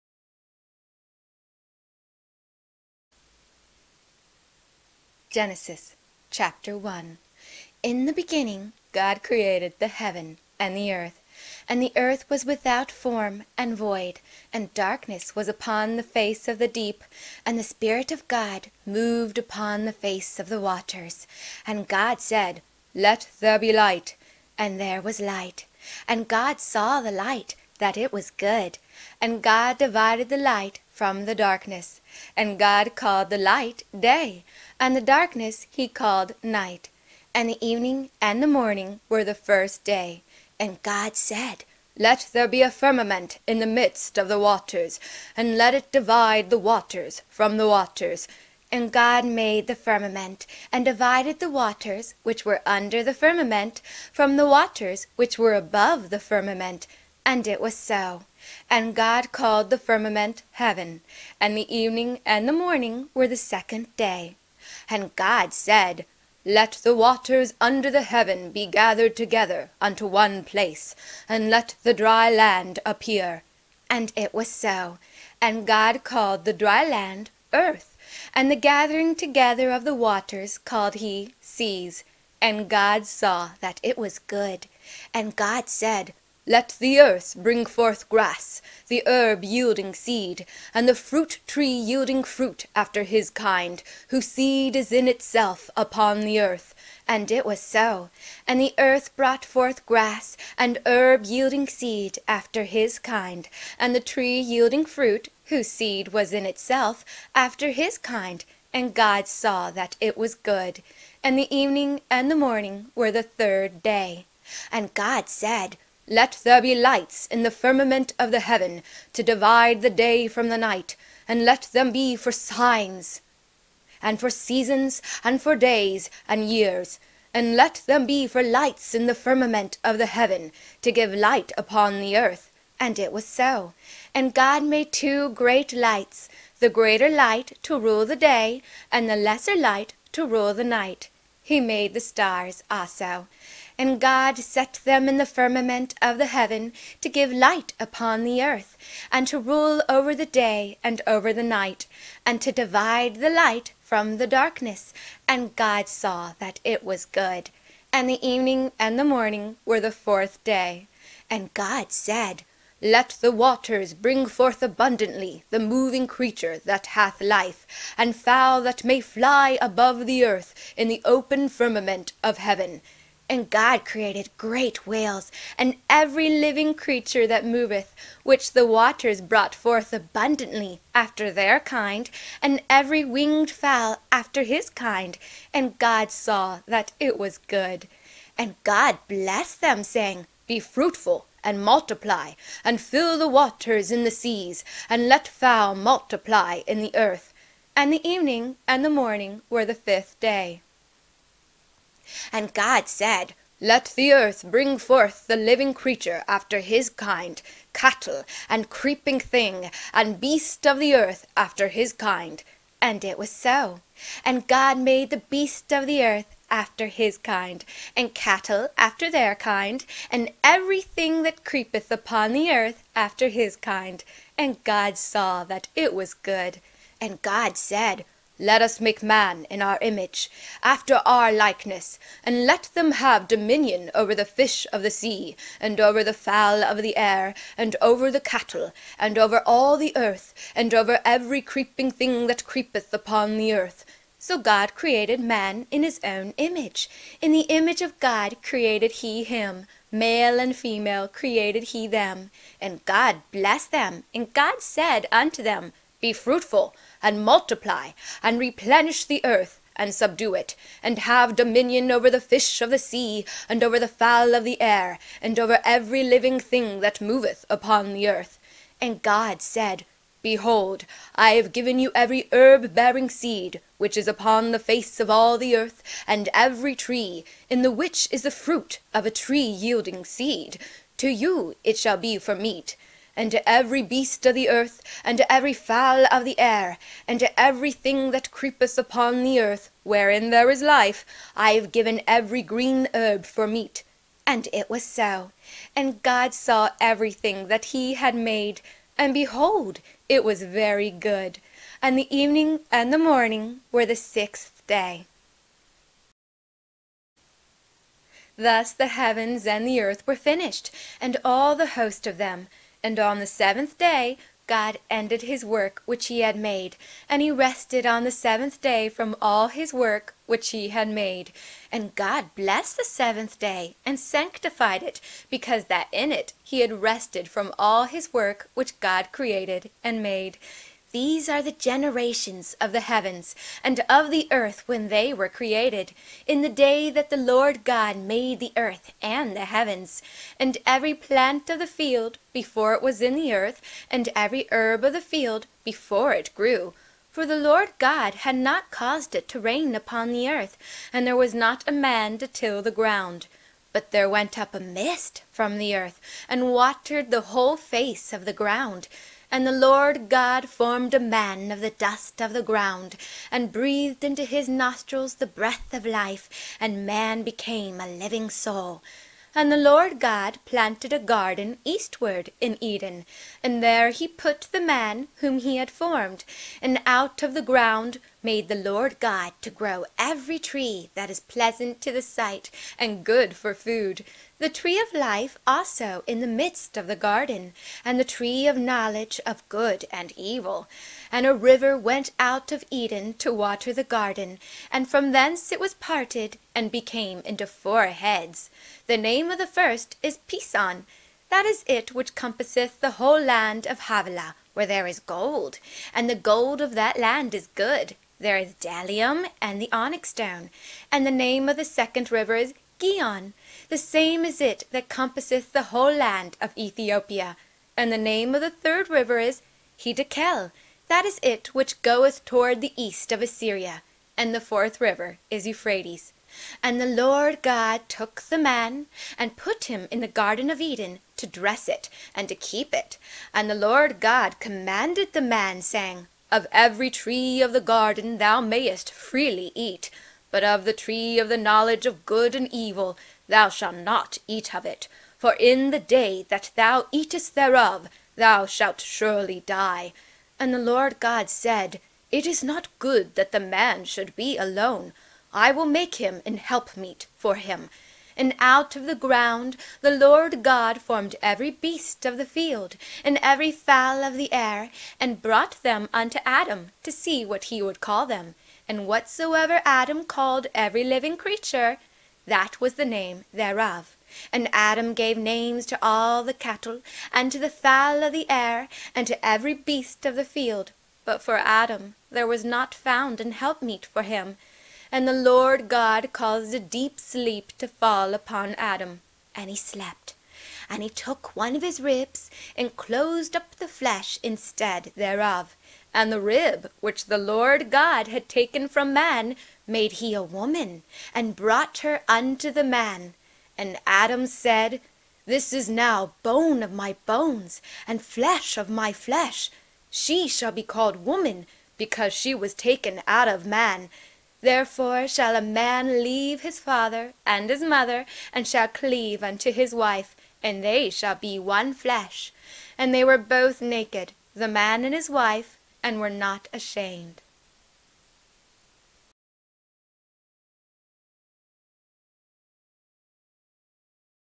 God Creates The World - Genesis 1&2 - Read by Stephanie J Yeager
God Creates The World
Genesis 1&2
Read by Stephanie J Yeager